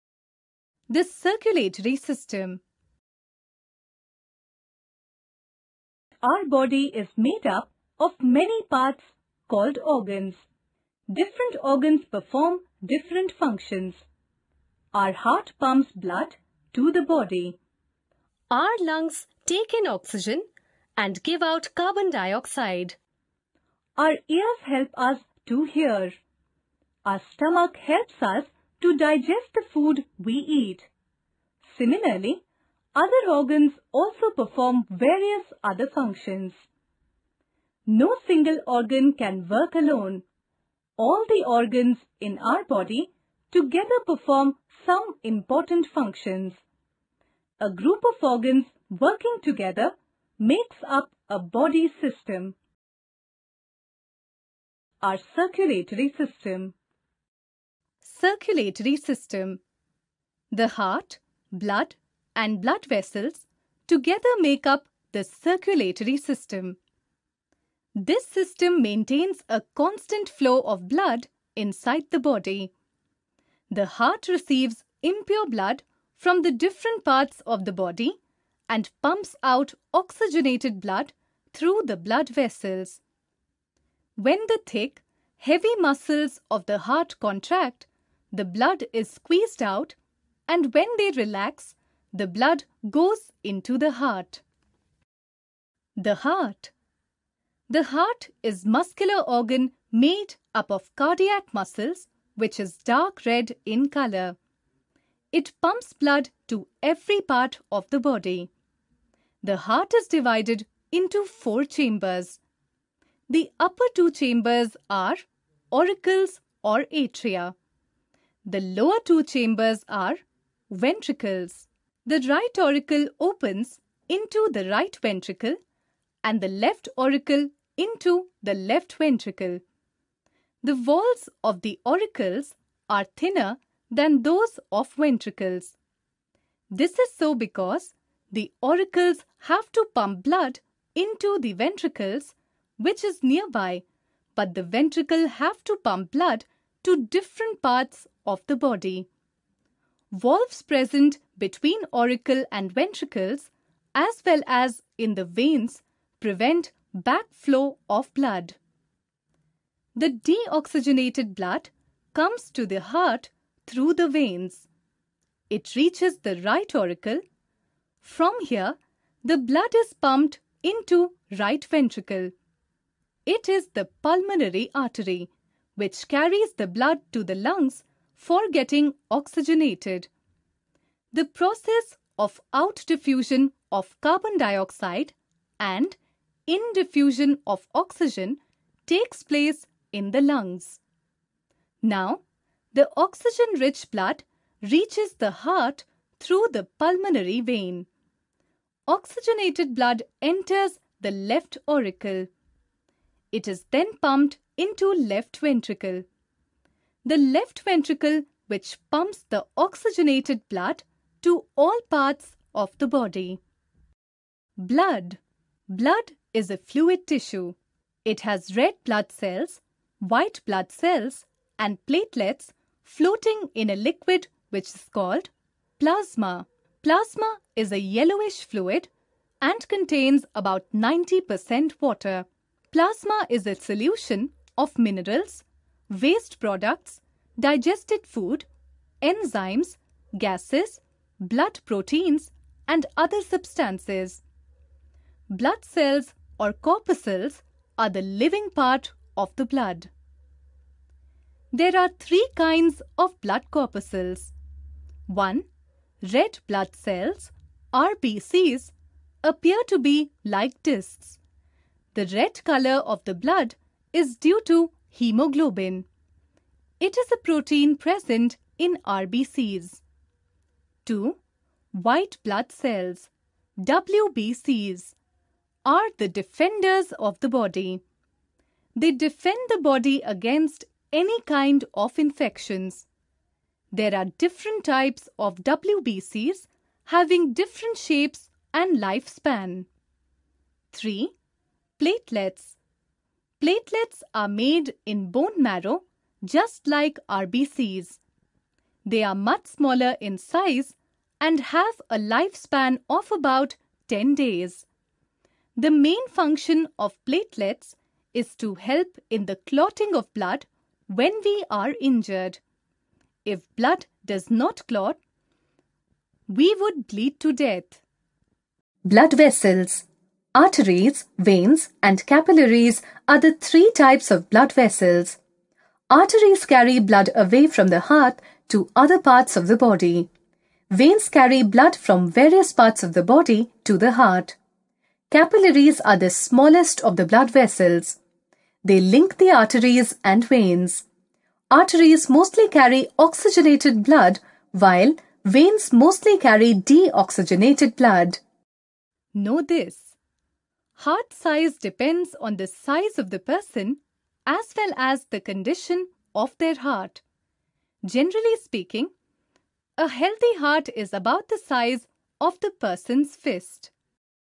The Circulatory System class-5
In this video from Kriti Educational Videos we get to know the circulatory system. We will know about Organs/Parts of the circulatory system, their structure, functions. We can also know about functions of blood. This video is suitable for class 5.
Contents:
1. The Heart
2. Blood
3. Blood vessels